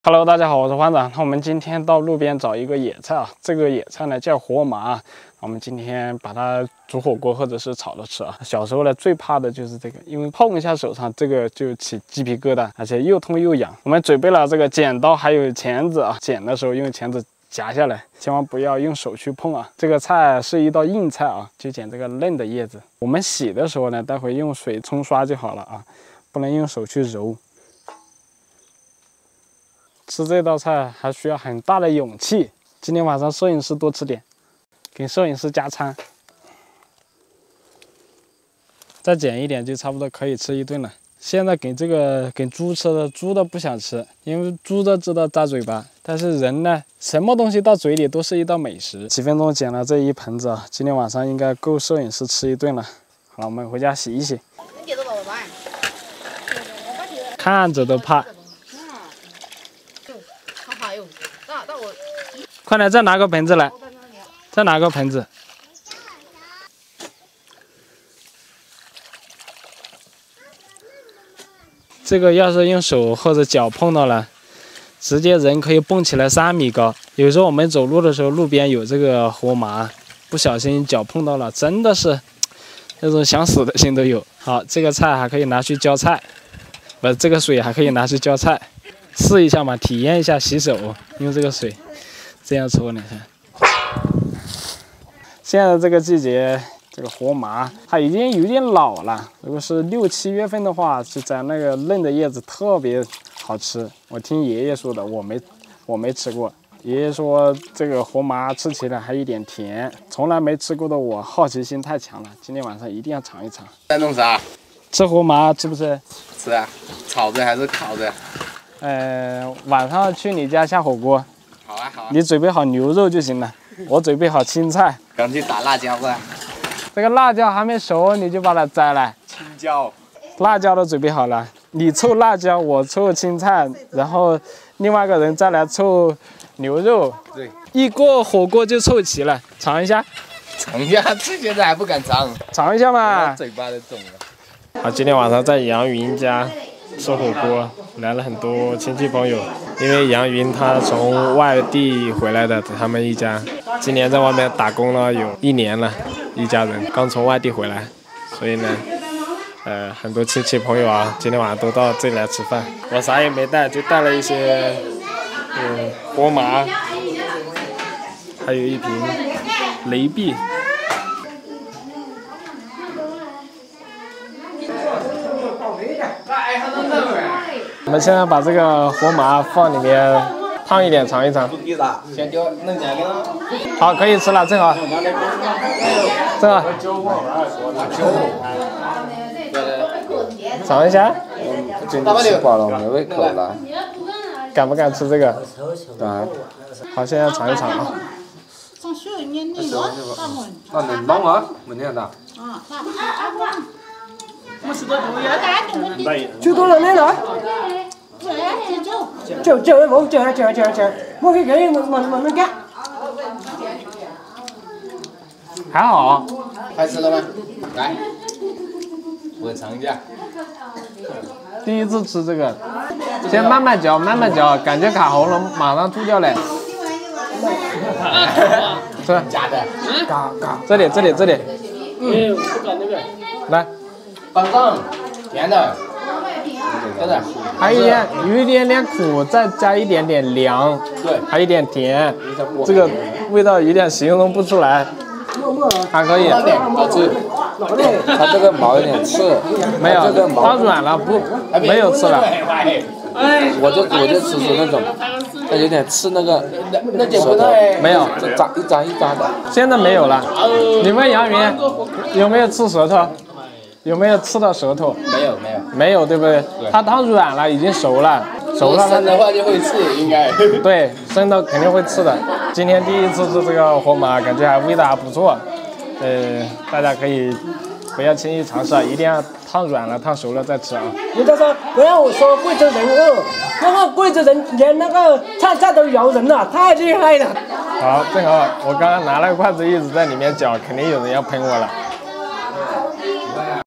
贵州农村发现新美食，这个野菜长满刺，碰一下叶子痛一天但很好吃
【欢子TV】
欢子TV，专注原生态农村题材的视频平台（原创）
分享生活中的点点滴滴
给每一位观众带来最朴实而又美好的生活